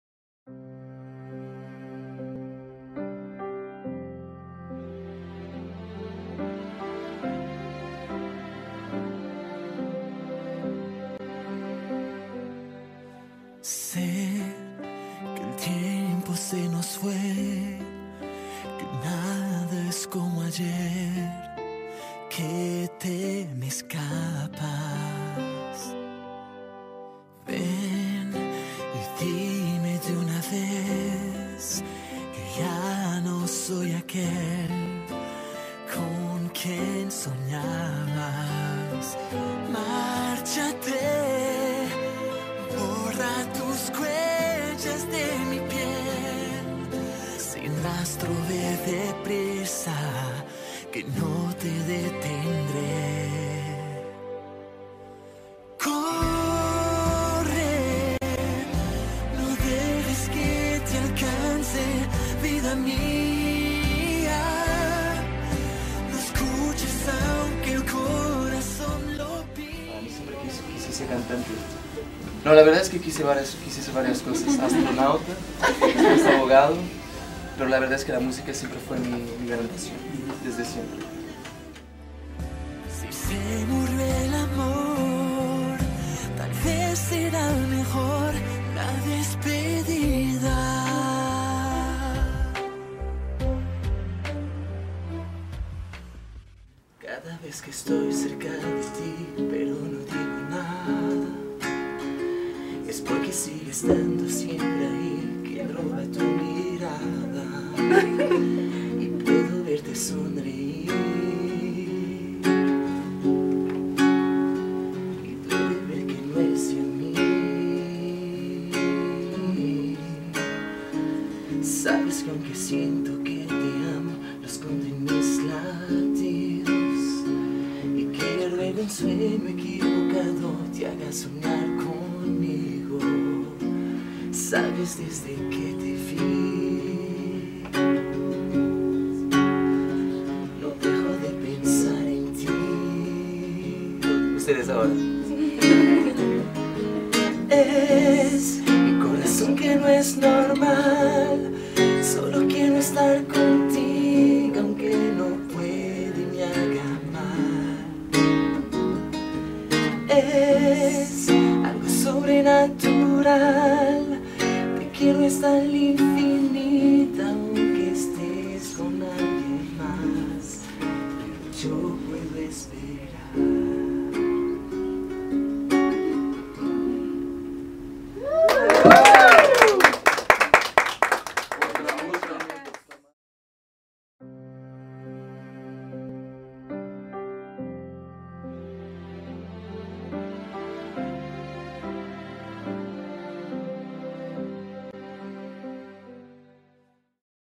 Mickael Carreira - Convivencia con fans
¡Mickael Carreira se reunió con fans en México y esto fue lo que paso!